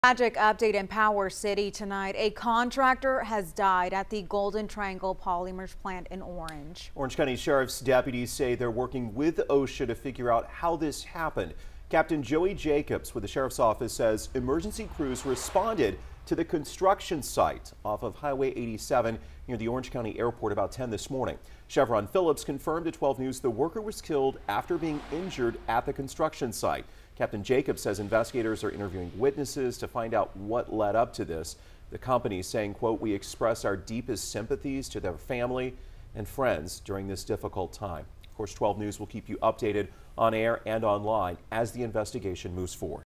Contract worker dies at Golden Triangle Polymers construction site in Orange County Friday morning
The worker's identity will not be released until their immediate family has been notified.